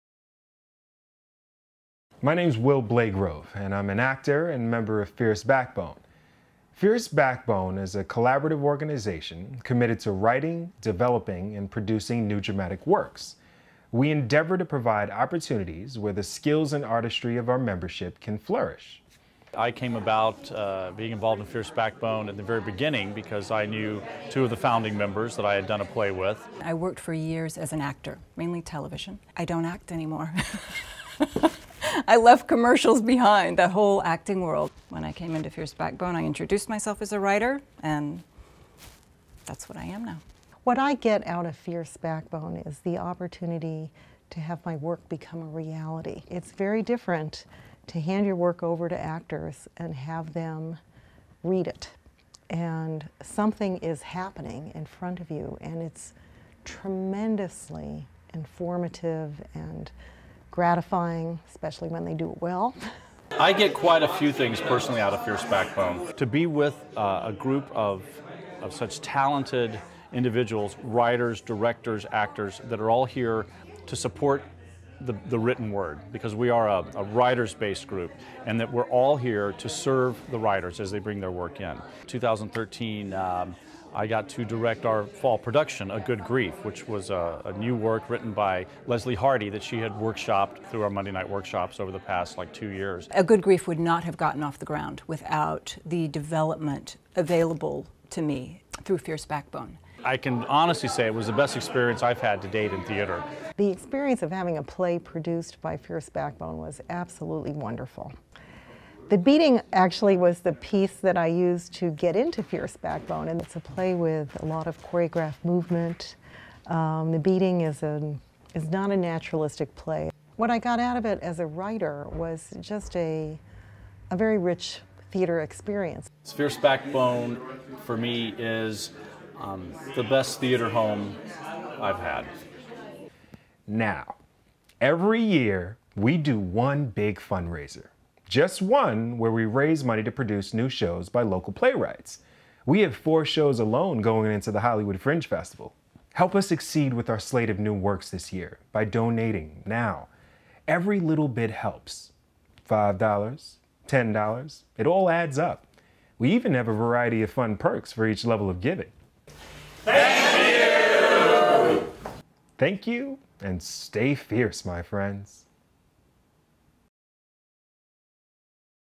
Fierce Backbone - Fundraiser Video
A video describing the great work Fierce Backbone does in the LA Theatre Community. -And a call-to-action, to support this organization financially.